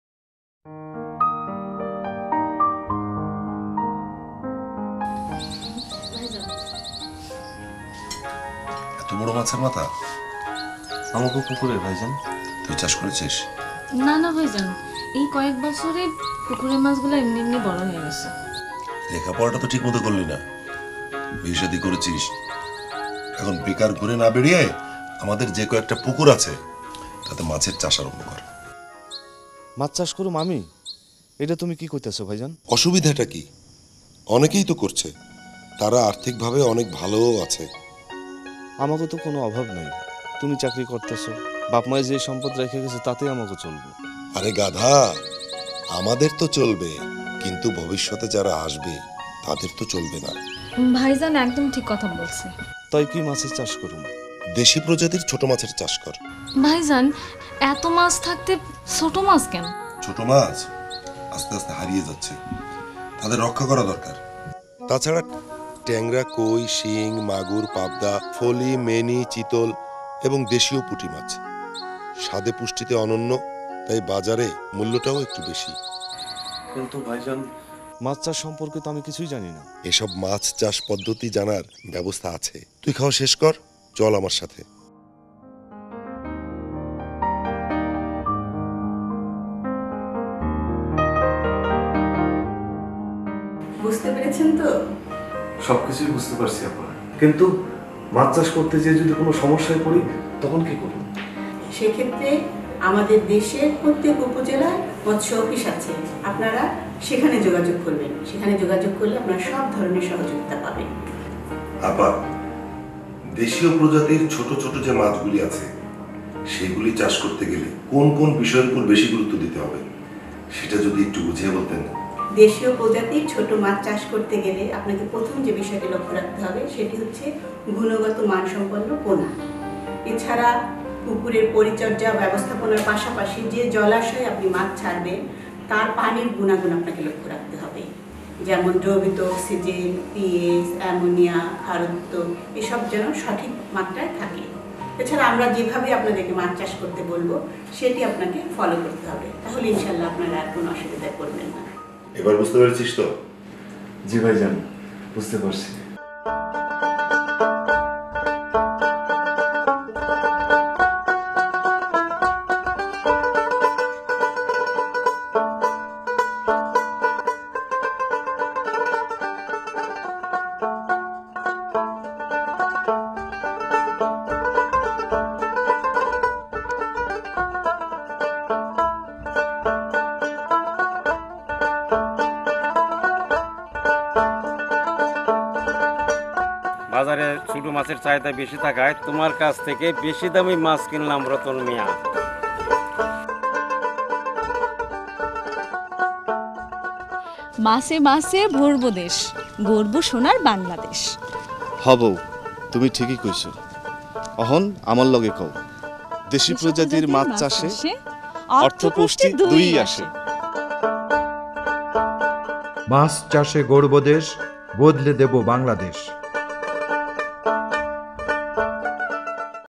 দেশী প্রজাতির মাছ চাষে অর্থ পুষ্টি দুই-ই আসে
দেশী প্রজাতির মাছ চাষে অর্থ পুষ্টি দুই-ই আসে, দেশী মাছ চাষ করুন, বেশি অর্থ ও পুষ্টির যোগান দিন।